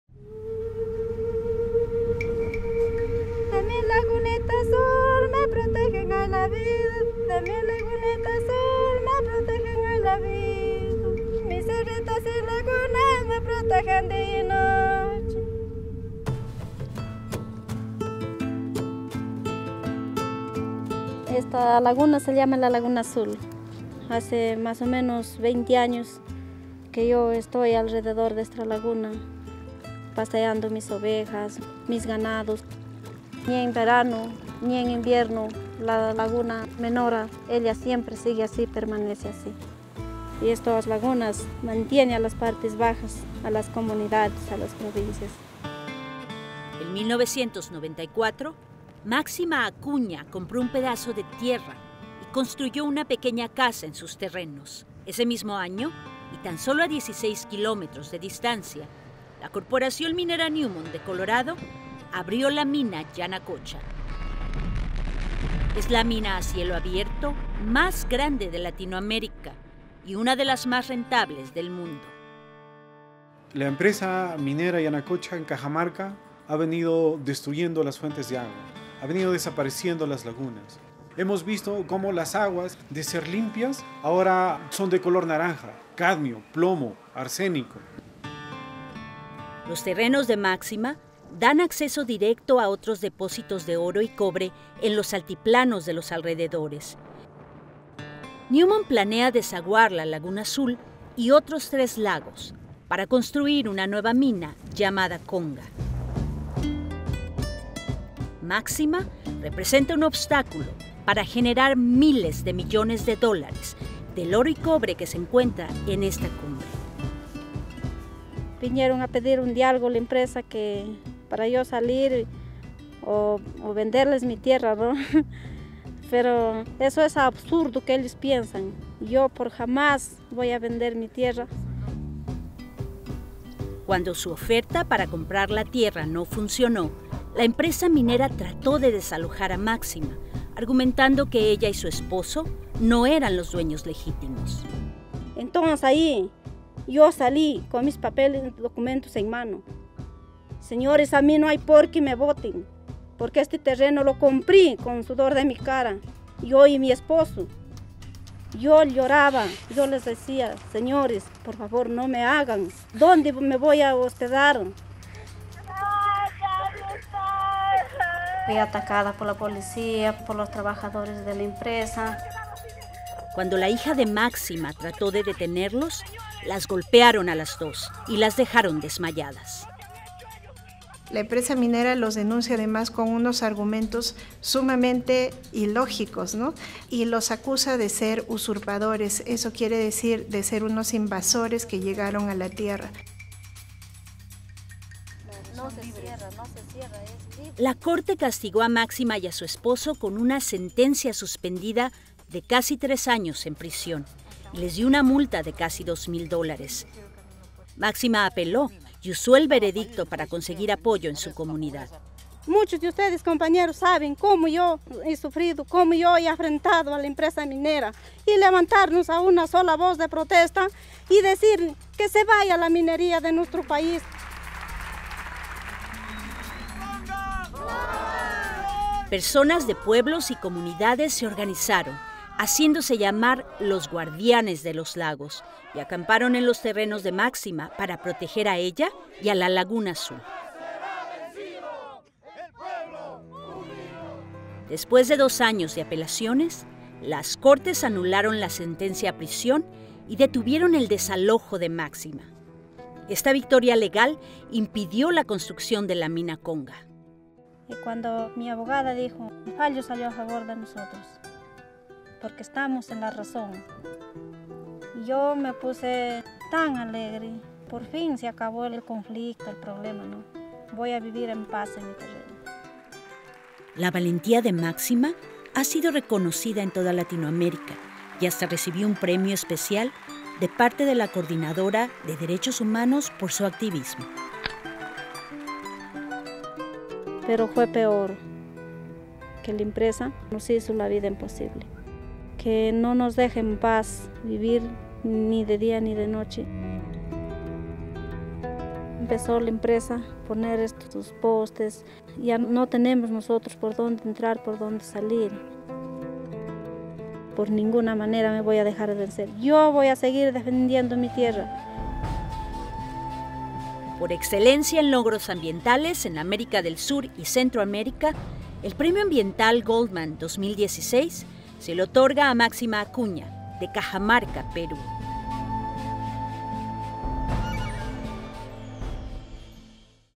Máxima Acuña, Galardonada del Goldman Environmental Prize 2016 para Sur y Centroamérica
Máxima Acuña, agricultora de subsistencia de la sierra norte del Perú, exitosamente defendió su derecho a vivir en paz y vivir de su propio terreno, un terreno codiciado por las empresas mineras Newmont y Buenaventura, impidiendo la entrada de la mina de oro y cobre, el Proyecto Conga, a una cuenca importante.